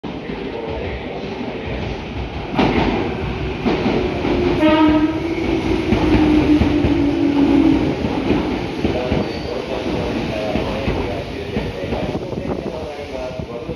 ●20190709 6500系 到着 ＠金山２
撮影路線：名鉄 名古屋本線
撮影駅：金山
行先：急行 金山
車両：6500系
両数：4両
撮影機材：【スマホ】SONY Xperia XZ2 Premium
MH：ーー
備考欄：